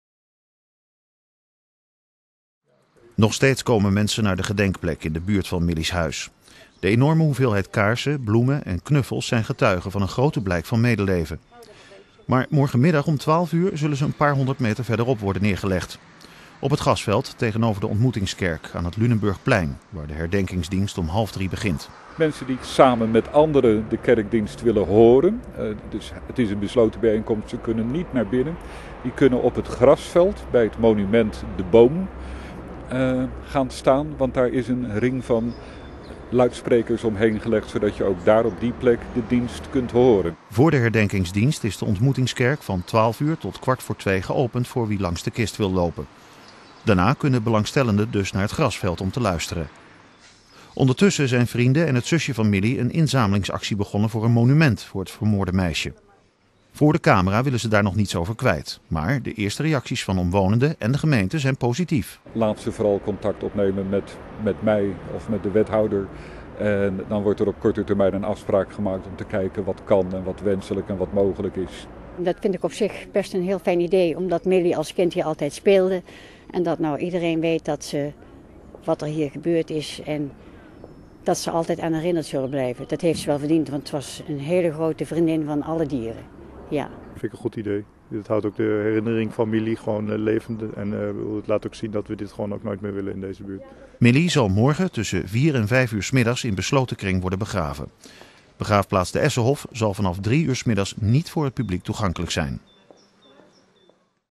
Monument voor Milly Boele
Familie en vrienden van Milly Boele zamelen geld in voor een monument voor het meisje, dat 2 weken geleden om het leven is gebracht. Milly zal woensdag 24 maart in besloten kring in Dordrecht worden begraven. Maar de familie wil buurtbewoners en andere belangstellenden toch de gelegenheid geven persoonlijk afscheid te nemen van Milly, daarom is de herdenkingsdienst buiten de kerk te volgen.